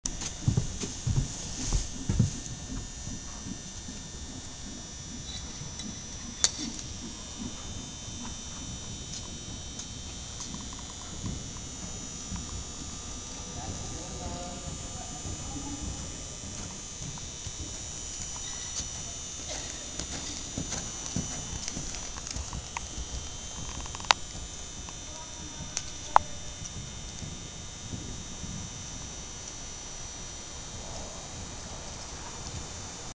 Marisa on Stevie
Marisa does a couple of warm-up jumps on Stevie.